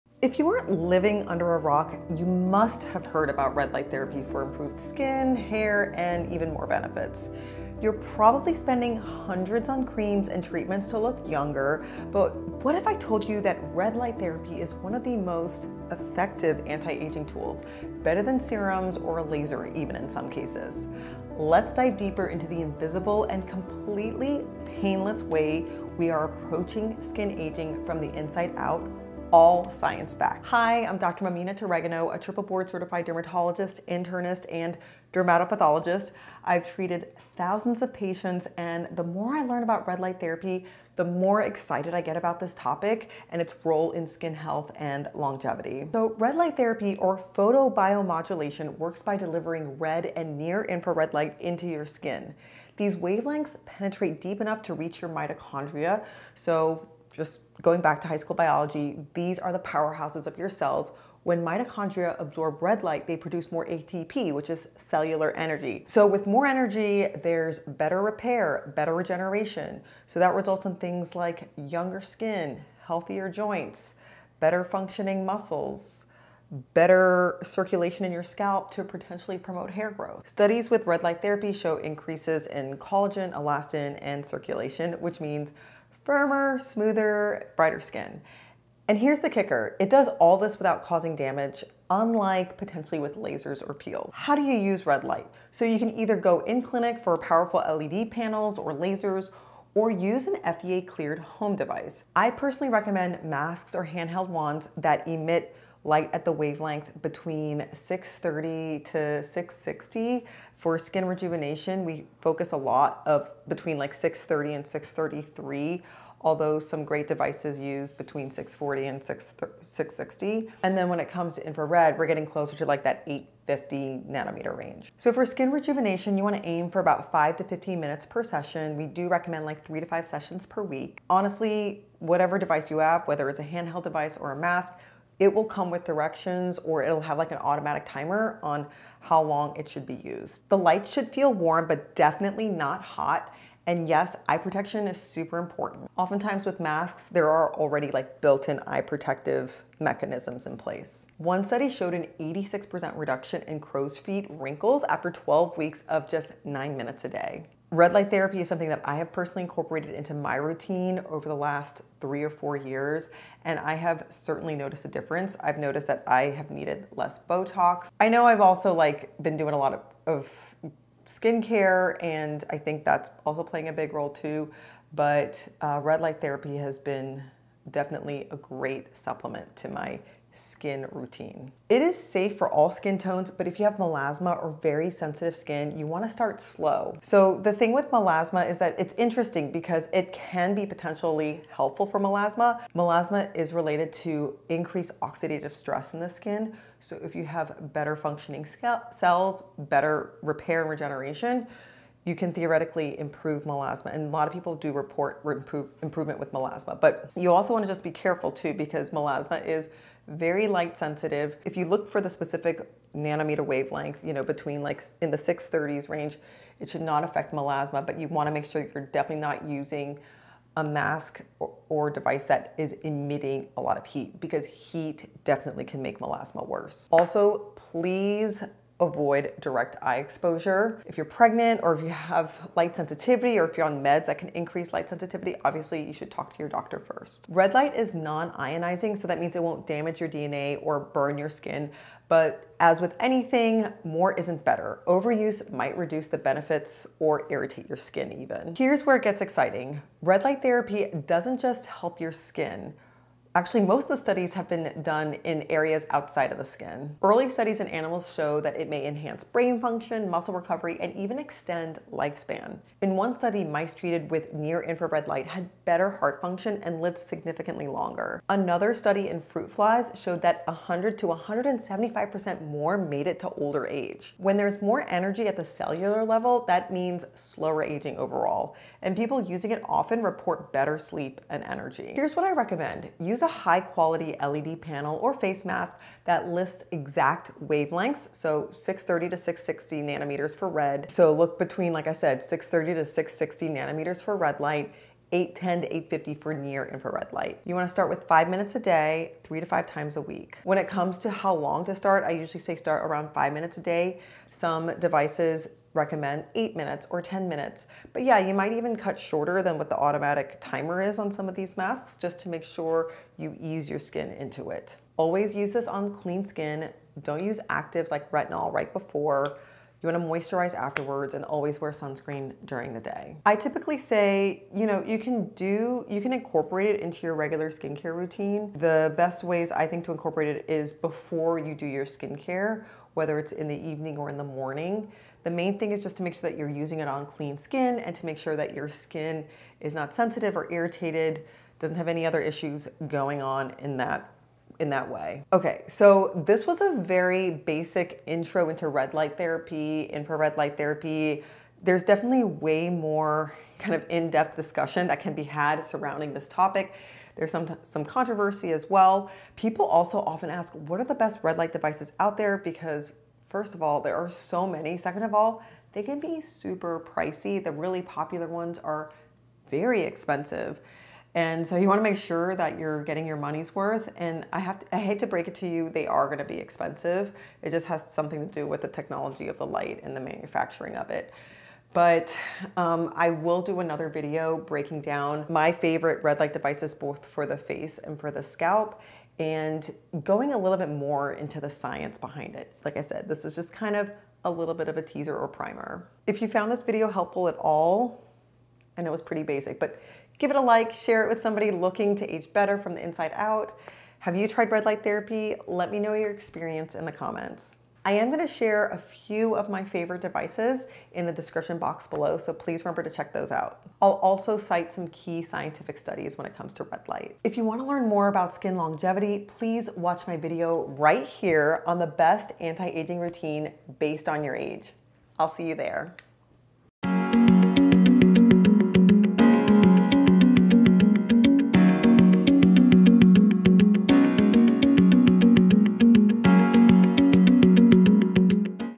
The Truth About Red Light Therapy for Skin (from a dermatologist)
Can red light therapy really reduce wrinkles, improve skin texture, and even help with hair growth? Or is it just another overhyped trend?

Hi, I’m Dr. Mamina Turegano, triple board-certified dermatologist, internist, and dermatopathologist and in this video, we’ll dive deep into what science actually says about red light therapy for skin health, anti-aging, and overall wellness.

We’ll cover:
- How red light therapy works on a cellular level
-  The difference between red light & near-infrared light
-  What the research really says about skin aging, collagen, and inflammation
-  At-home devices vs. in-office treatments — what’s worth it
-  Safety tips before you start

Whether you’re curious about fine lines, skin barrier health, or longevity, this video will help you separate fact from hype.

More Science-Backed Skin Advice:

📄 A few of the references of the research I mentioned in the video: